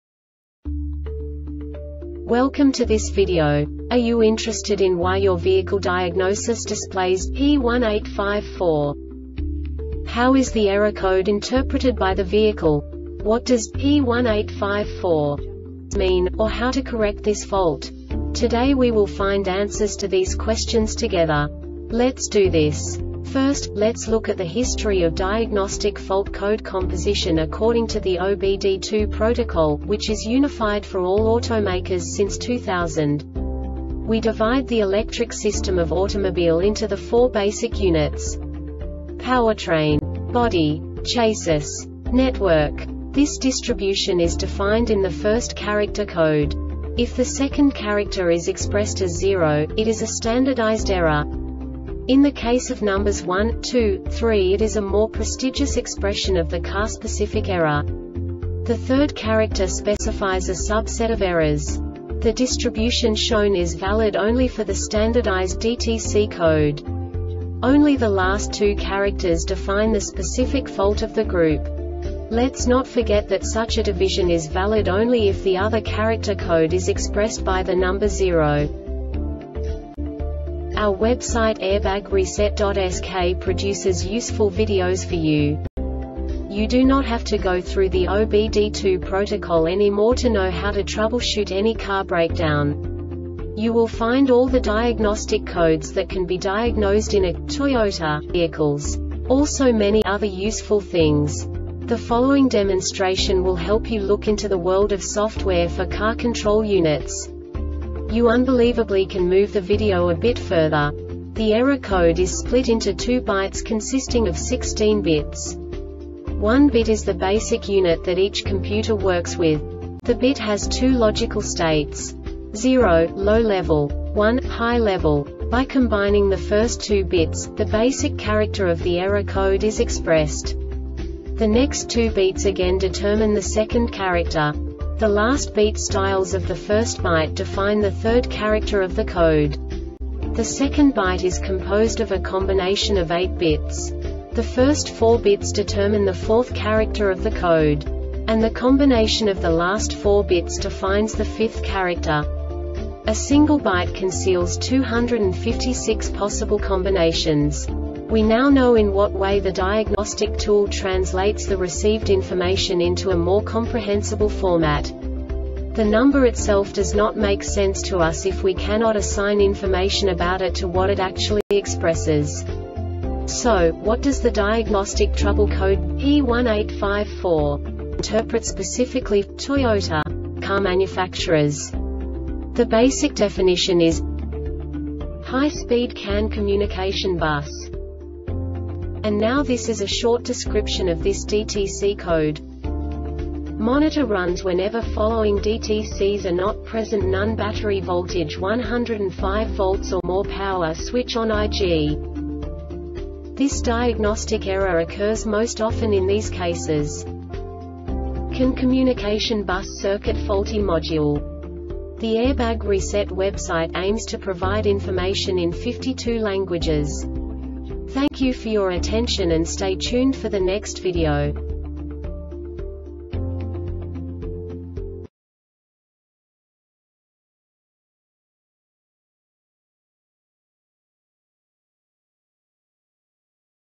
DTC Toyota P1854 Short Explanation
Contents:
0:21 Basic DTC analysis according to OBD2 protocol standard.
1:48 Insight into programming
2:58 A diagnostically understandable description of the Diagnostic Trouble Code
3:05 Basic error definition

Make:
Toyota
Code:
P1854
Definition:
High Speed CAN Communication Bus
Description: